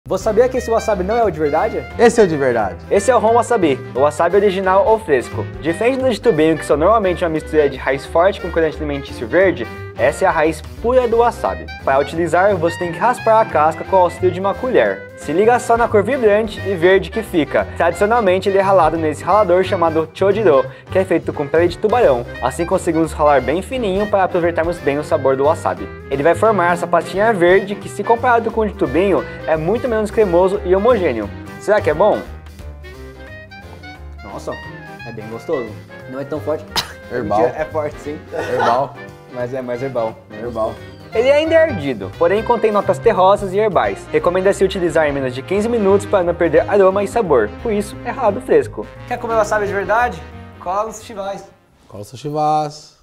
WASABI VERDADEIRO, SÓ PARA OS FORTES
Quer dizer que eu vivi uma mentira durante a minha vida toda??!

O wasabi original, chamado de HON WASABI, é muito diferente da pasta verdinha vendida em tubinhos. O Seidi provou esse wasabi ralado fresquinho com o Vaz, do Sushi Vaz!

🗣 ️ Comente aí se você já conhecia esse wasabi de verdade!